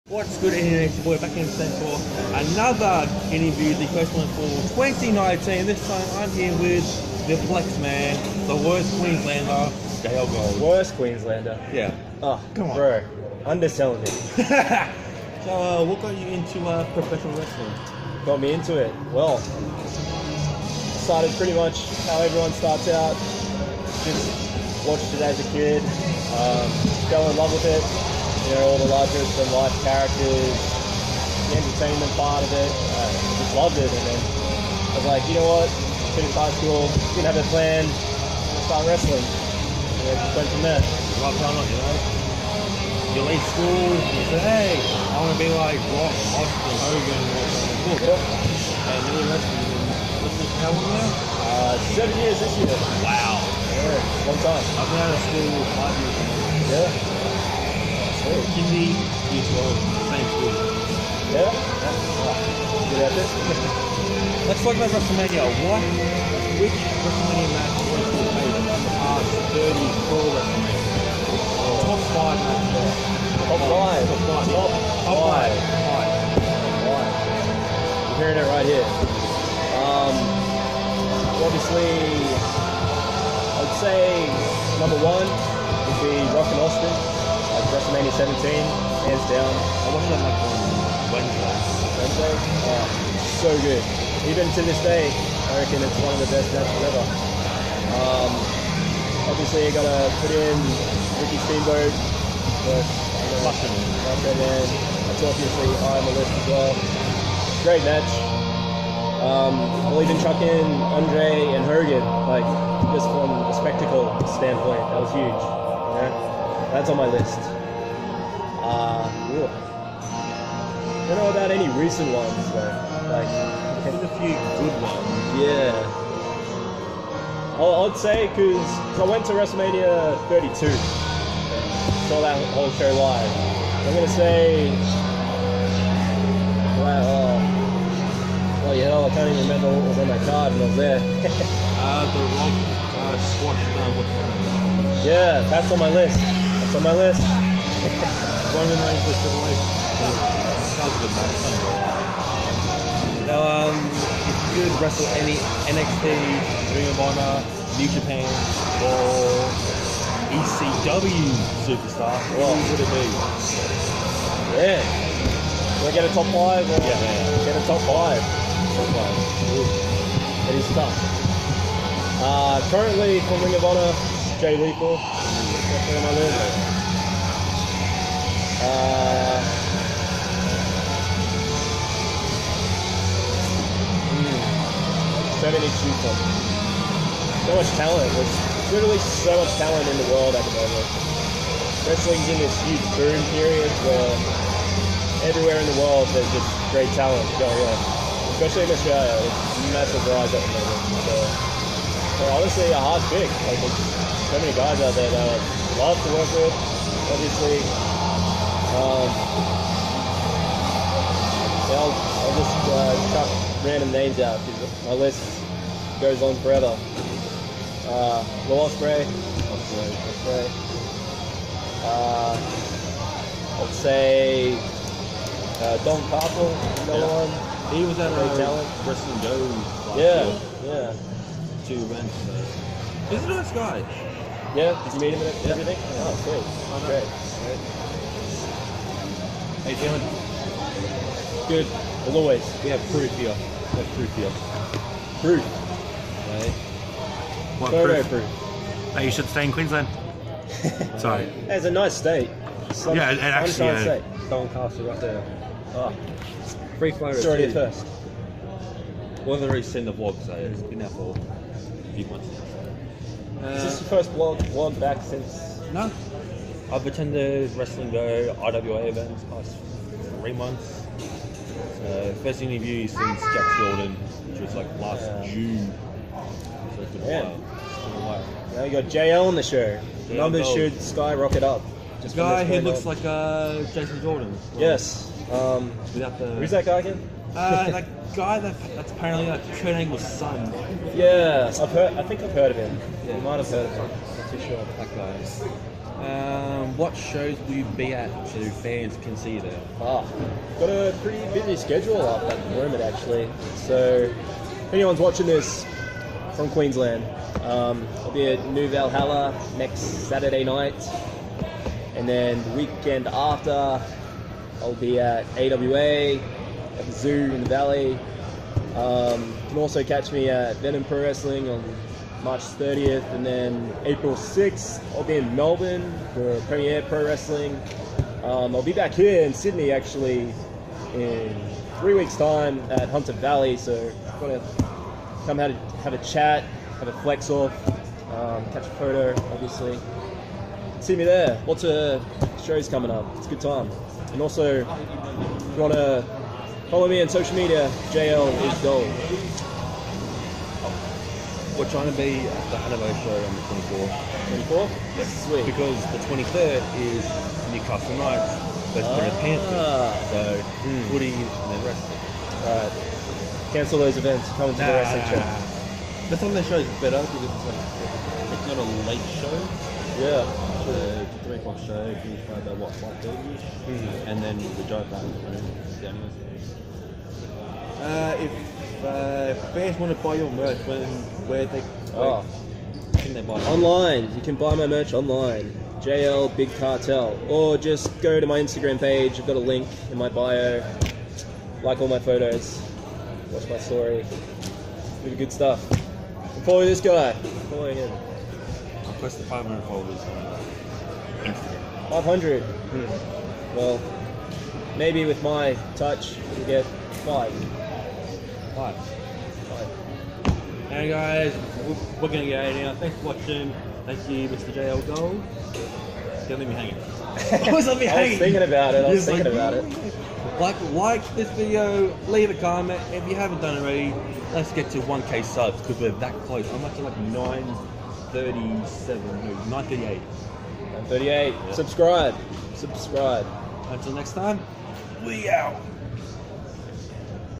JL Gold Interview//March 1st 2019
What's Good everyone welcome back to the channel and welcome back to the first Interview for 2019 today I sat down with One Of Queensland's Finest Wrestlers JL Gold the guy who looks like Jason Jordan from WWE. We talked about what got him into wrestling and his top 5 WrestleMania Matches and his top 5 Dream matched he would like to face..